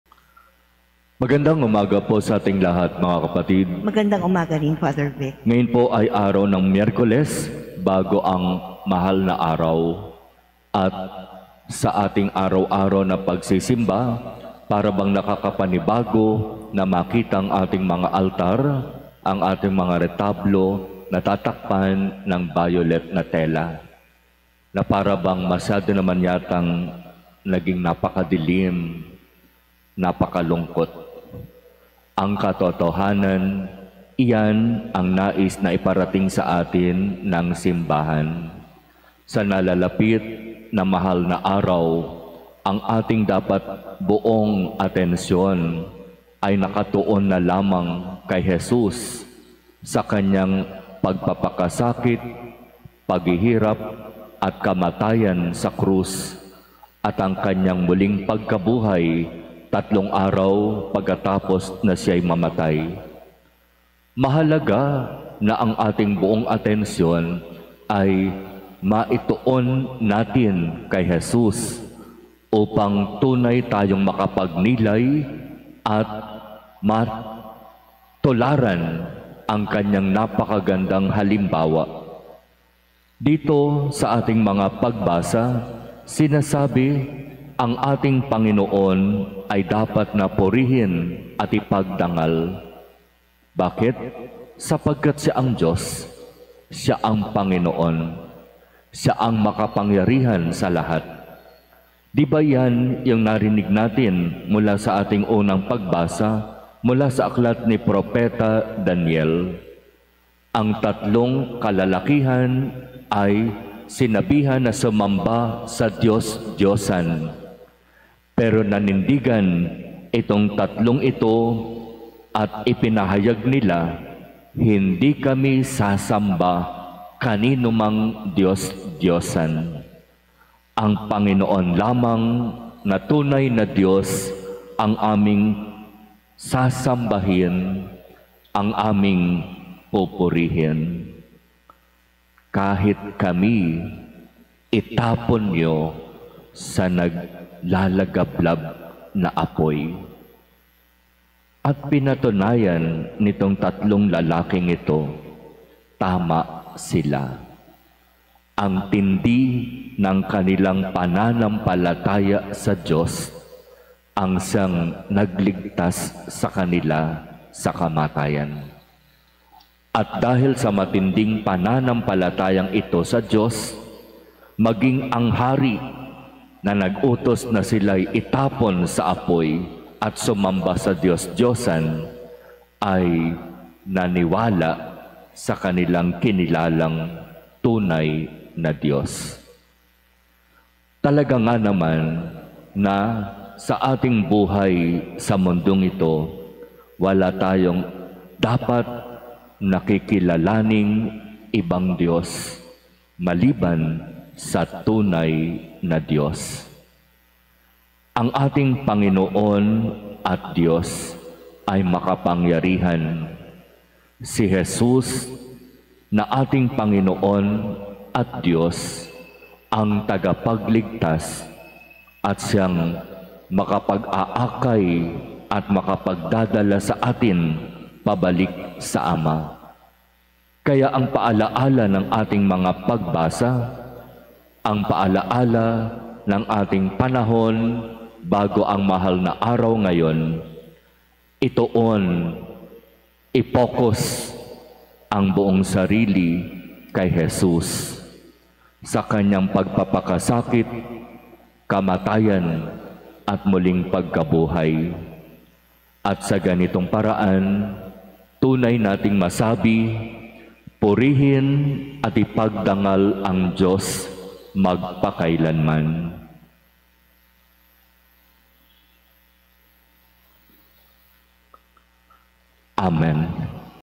SabiNiFather - March 20, 2024 | 6 AM Mass
SabiNiFather - Homily of Fr. Vic Flores Jr on the Wednesday of the 5th Week of Lent - 6:00 AM Mass | March 20, 2024

Jesu Christi Passio sit semper in cordibus nostris (May the Passion of Jesus Christ be always in our hearts.)